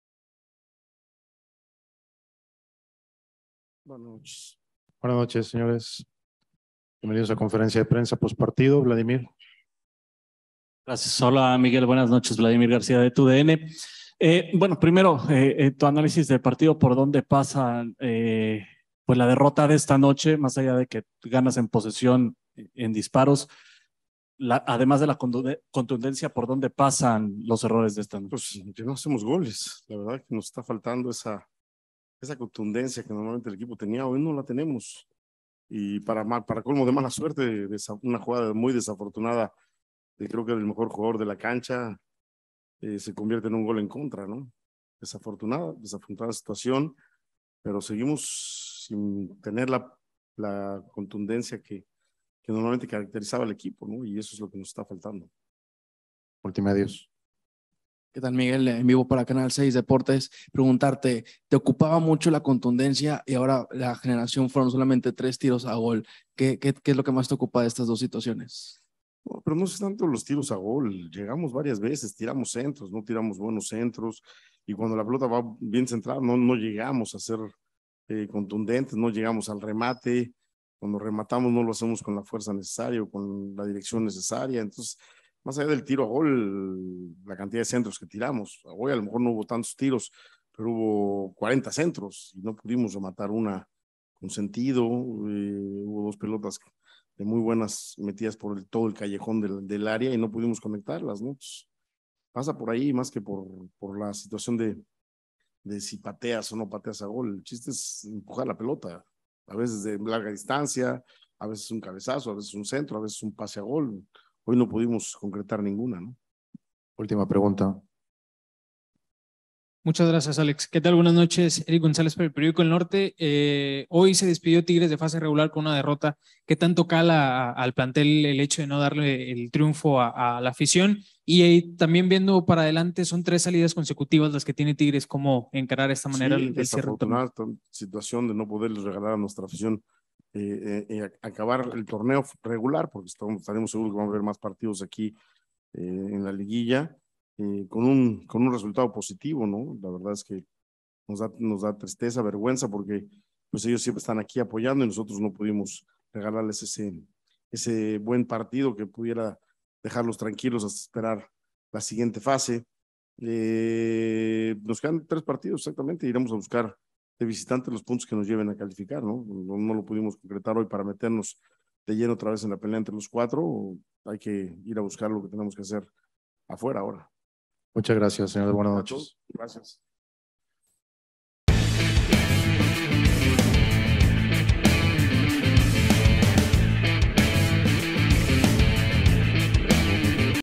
Miguel Herrera "No hacemos goles, nos está faltando es contundencia que normalmente el grupo tenía"
Miguel Herrera habló en conferencia de prensa al término del partido que su equipo cayó en casa ante León
"No hacemos goles, nos está faltando es contundencia que normalmente el grupo tenía"
"No es tanto los tiros a gol, llegamos varias veces, tiramos centros y no llegamos al remate,"
"Desafortunadamente no pudimos regalarle a la afición un resultado positivo, la verdad es que nos da tristeza y vergüenza "